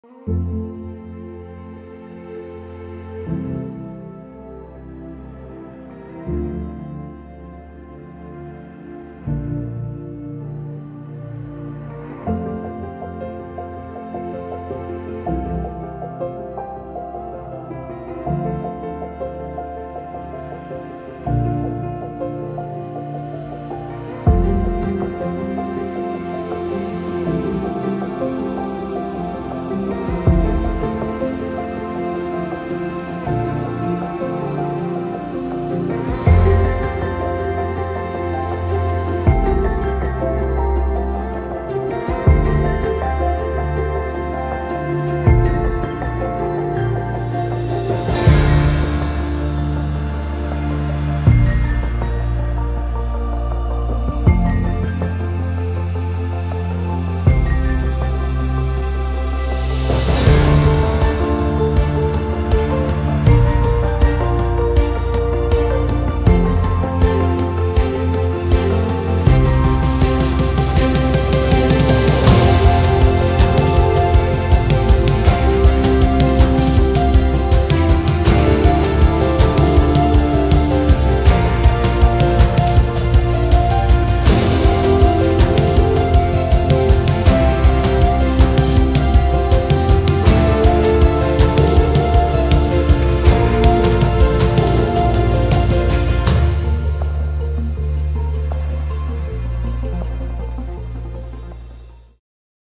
法鬥包子的弟弟菜包 漫步在四號公園跑跑寵物寫真
Map影像團隊歡迎合作
電商平台商品形象拍攝 活動紀錄 微電影MV  CF廣告拍攝 等

hiphoppd@yahoo. @